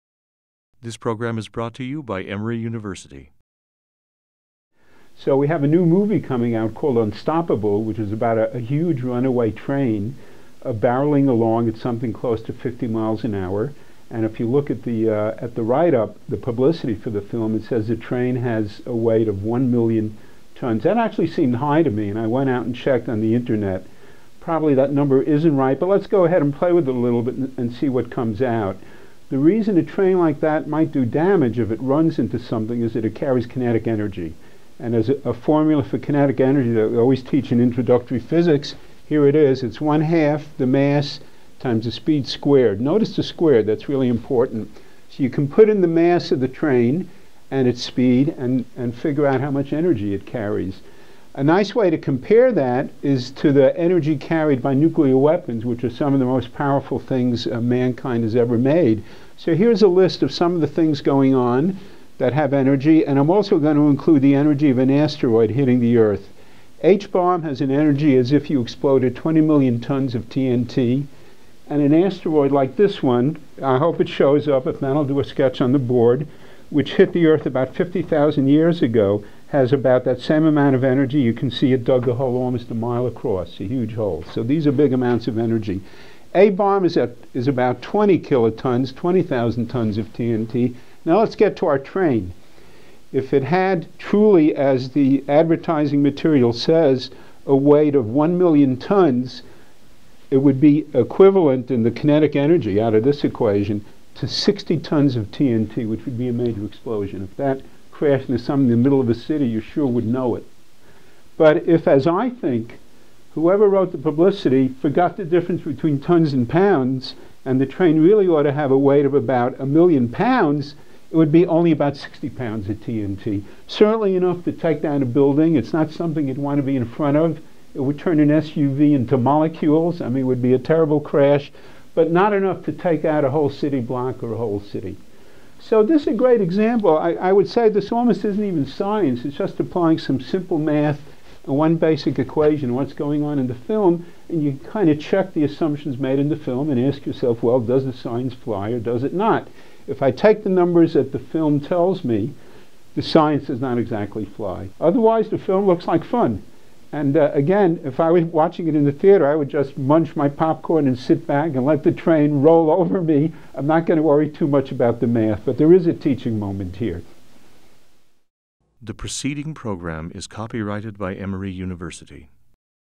How Powerful is Unstoppable?
How realistic is the science and physics behind the new movie Unstoppable? How much power does that unstoppable train really have?  Emory professor Sidney Perkowitz discusses all this and more as he looks into the true power of Unstoppable.